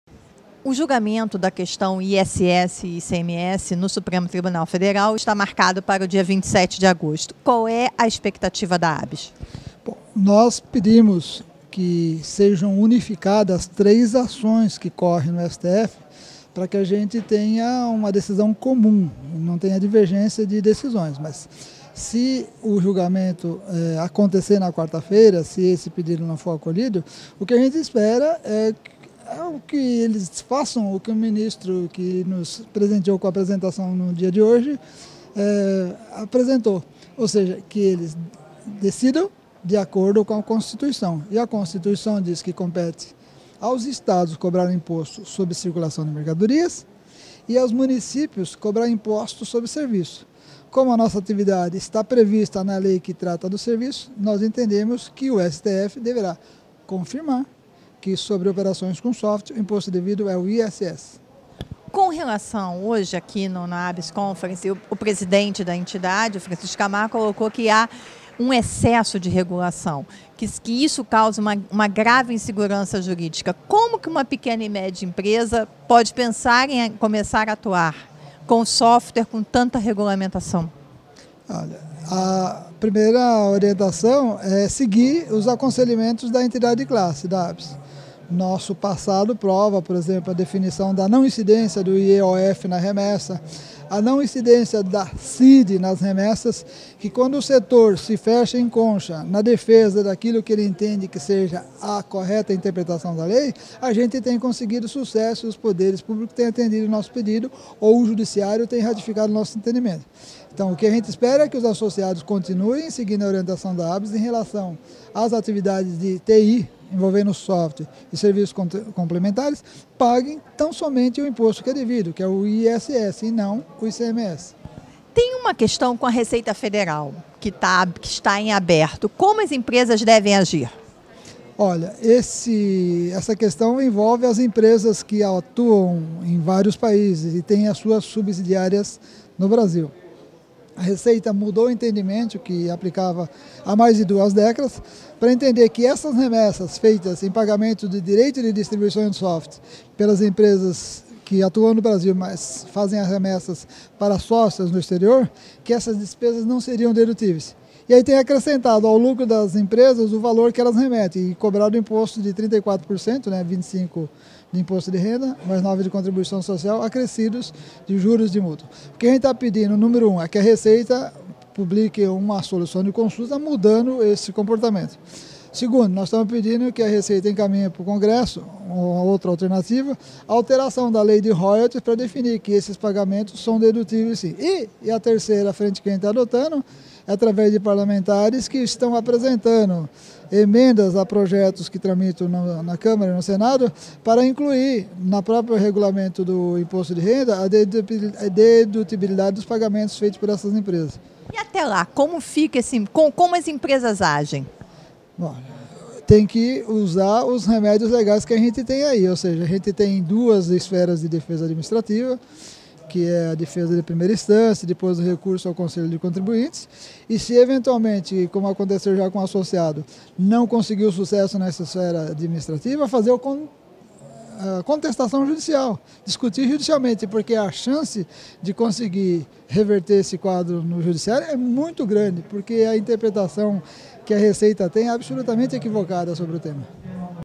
Empresas devem usar 'remédios legais' na disputa com a Receita Federal
Fisco mudou entendimento de duas décadas e mudou a cobrança de tributos, onerando as multinacionais em 34%, informa o diretor Jurídico da ABES, Manoel dos Santos. Sobre ISS/ICMS, ABES quer uma decisão comum do STF para evitar divergências.